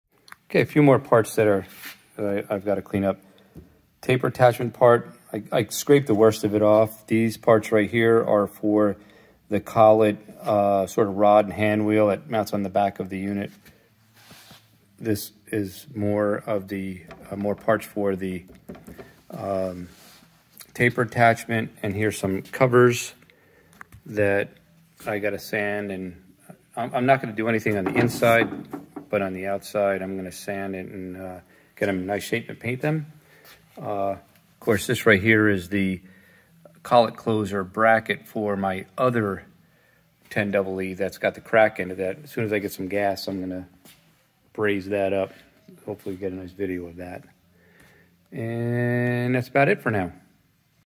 Monarch 10EE lathe... A little Ramblin
So guys... I'm traveling this week so I won't be posting a super deluxe video! Here's just a little snippet of some items I have to work on as part of cleaning up my Monarch.